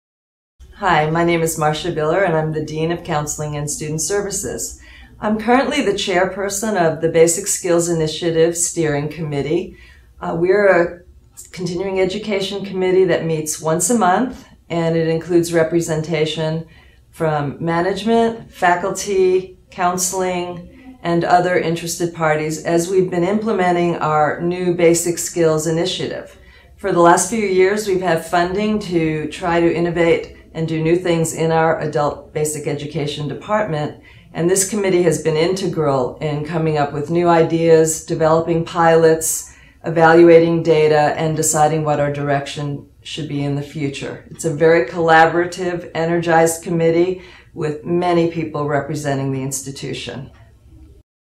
Marcia Biller, Basic Skills Initiative Steering Committee
Marcia talks about the BSI Steering Committee